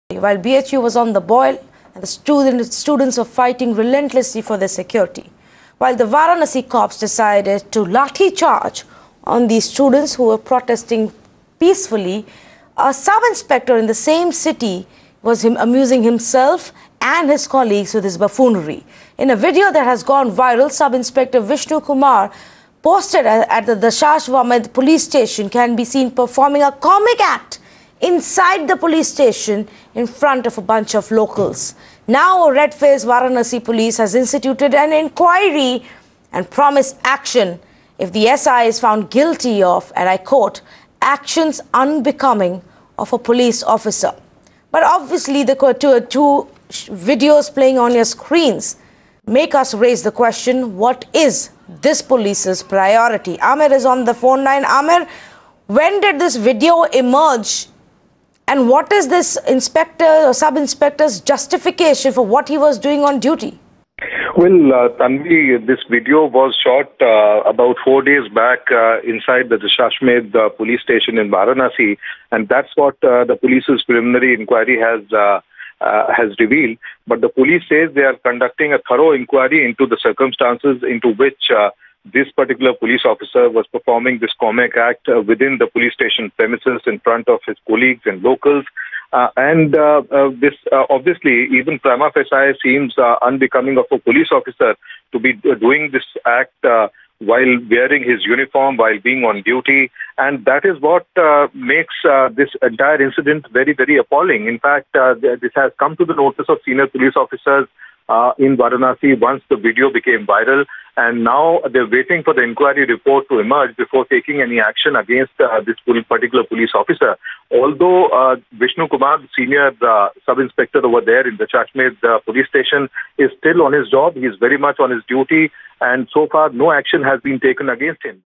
देखें, BHU में तनाव के बीच एक पुलिसकर्मी कर रहा है अपने सहकर्मियों का मनोरंजन
बनारस हिंदू विश्वविद्यालय में जारी तनाव के बीच एक पुलिसकर्मी अपने सहकर्मियों का मनोरंजन करते वक़्त कैमरे में क़ैद हो गया।